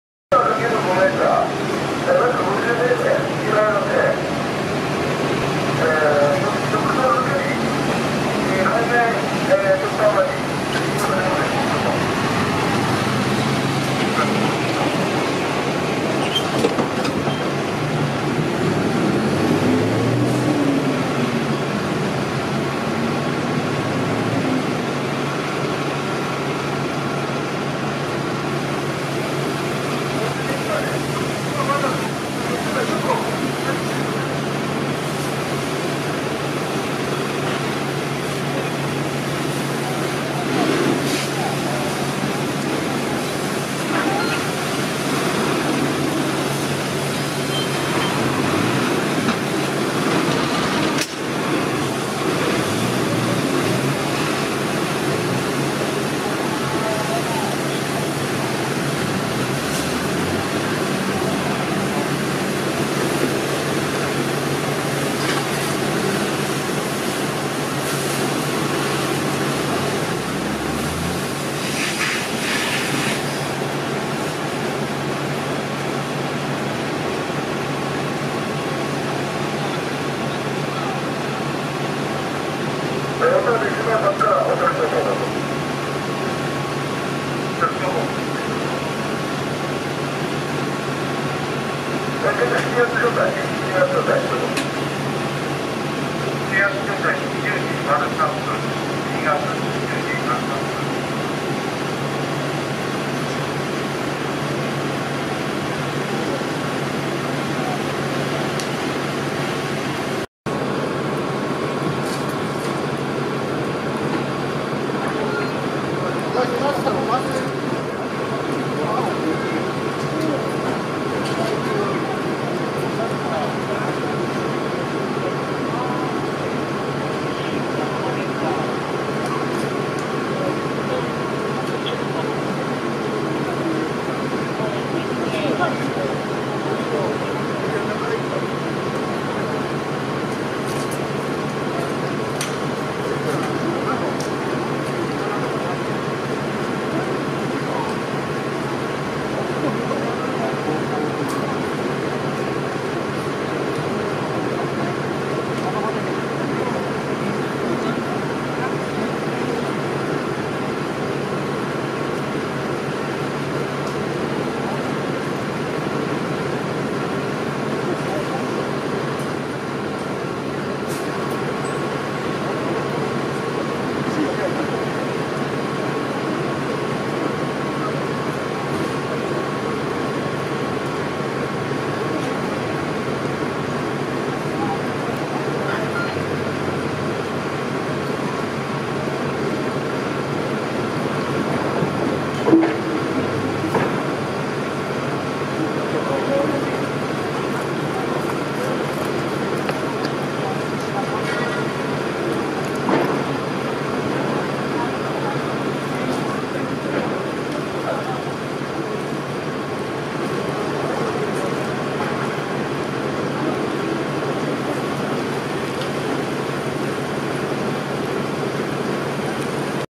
小樽市ヴェネツィア美術館前で火災！
サイクリング中に火災に遭遇しました。